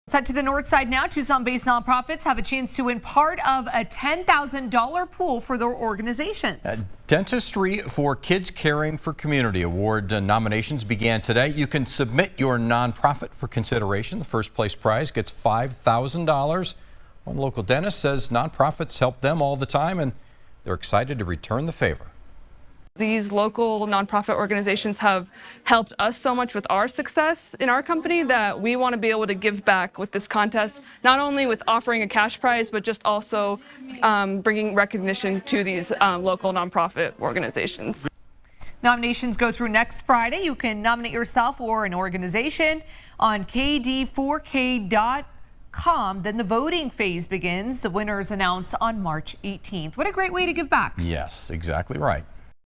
Tucson nonprofits can nominate themselves to win up to $5,000
General Dentistry 4 Kids opened up nominations for their "Caring4Community" award